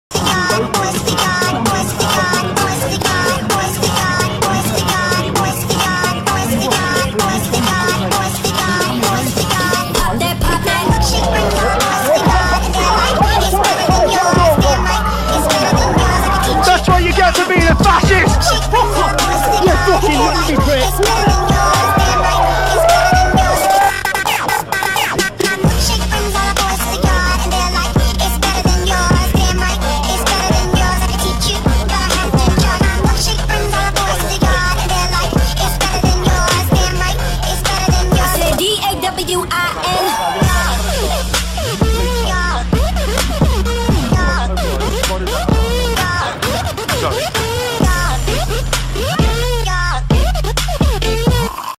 Milkshakes Against Fascism: Nigel Farrage and Tommy Robinson try the “Milkshake Challenge”
Creamy droplets of sugar infused milk rains lately upon racist and fascist candidates across Europe, ahead of the European parliament elections that take place between 23 and 26 May 2019.

“Flying” milkshakes have already been served to racist politicians several times this week alone in Britain, like in the case of  Nigel Farrage, former leader of far-right UKIP party and current leader of the Brexit Party, Carl Benjamin, a UKIP candidate and Tommy Robinson, former leader and founder of the fascist EDL party, who has recently received a flying milkshake more than once.

The “Milkshake Challenge” has also spread to Sweden, while in Malmö local fascist Dan Park also got a flying milkshake served as a means to improve his image.